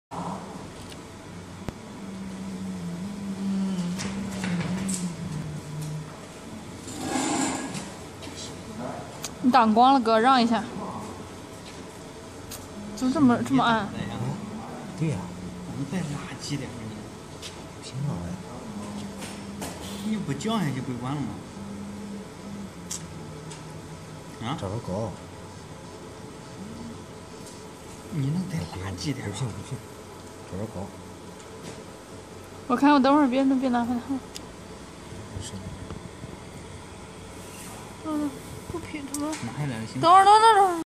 Mark on surgical instrument by fiber laser marking machine ------ OPTIC TECH
Optic Co. are specialized in laser marking machine,laser cutting machine and laser welding machine for 13 years.